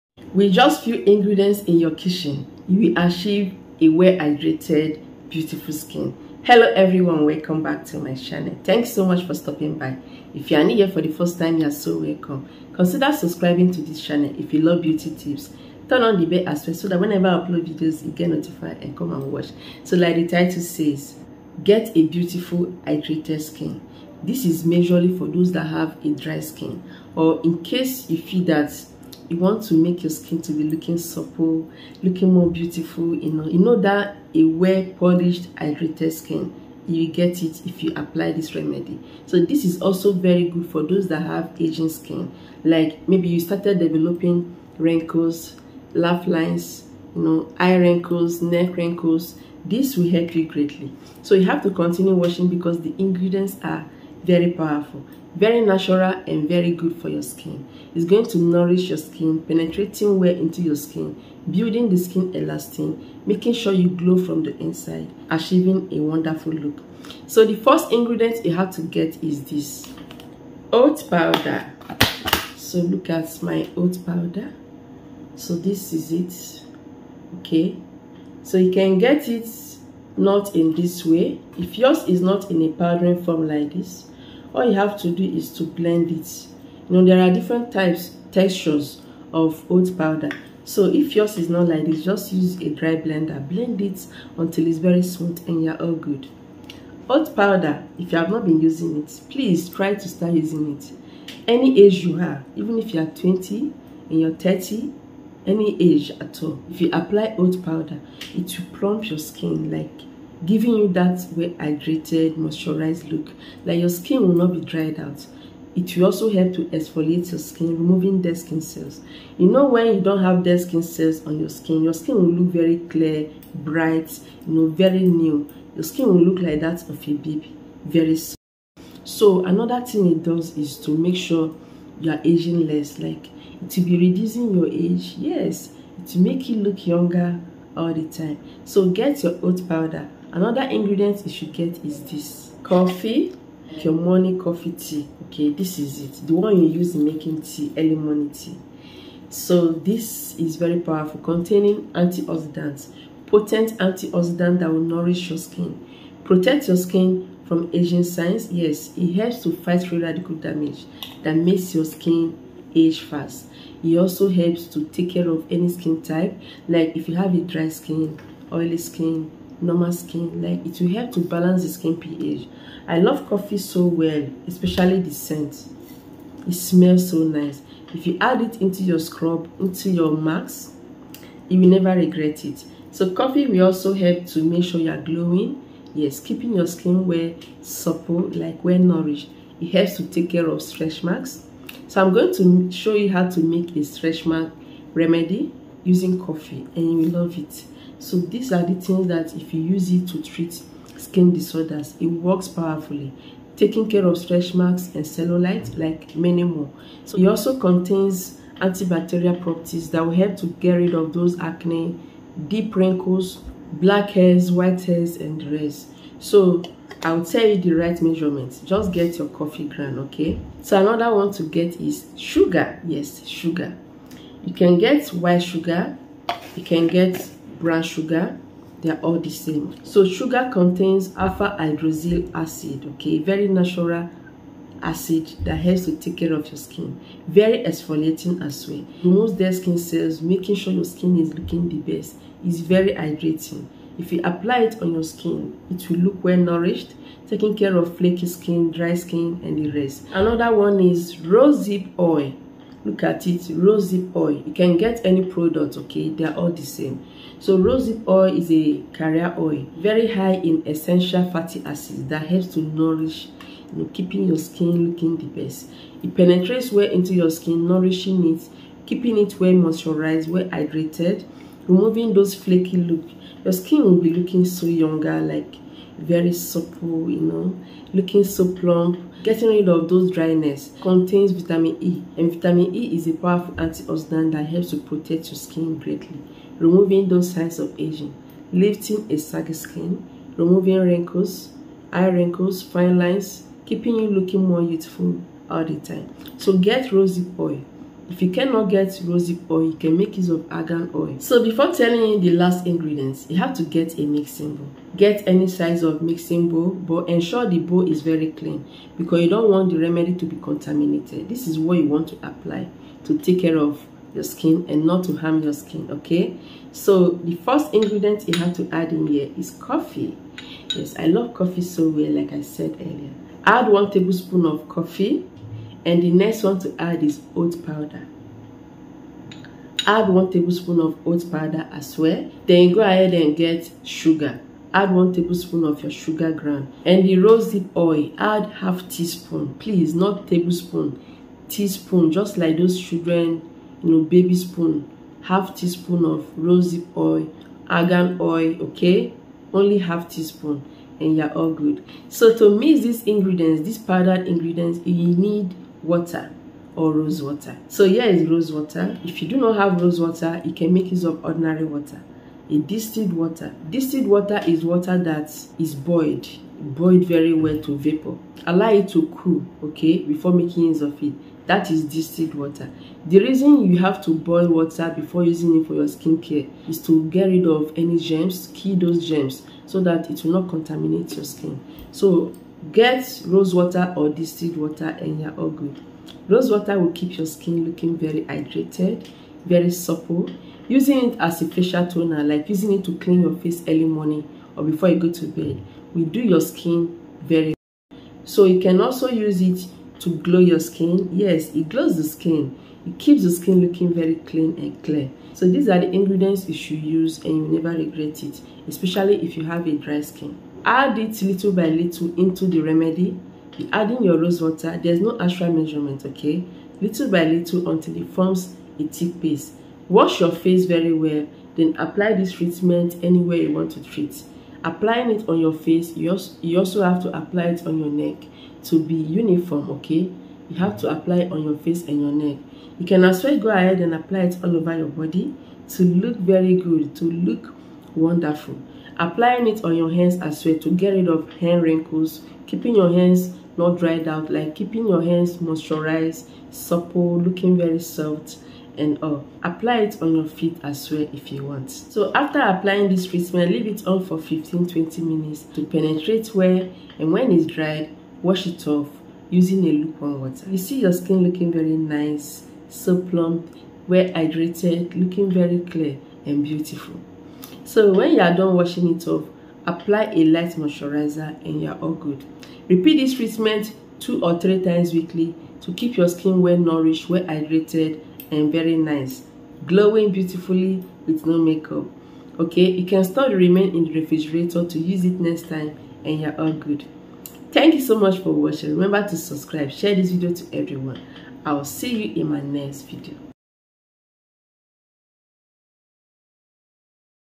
GET BEAUTIFUL HYDRATED SKIN WITH INGREDIENTS IN YOUR KITCHEN | REMOVE DRY SKIN
Welcome back to the Pamax NaturalTips channel and new beauty tips and hacks. Today you'll know how to get clear, smooth and glowing skin naturally at home. I'll show you how to hydrate your skin from natural ingredients that you can easily find at your kitchen. We use oat powder, coffee and vitamen E. Apply this remedy and your skin will be very smooth and hydrated. No more dry skin! Watch and find out more in this video.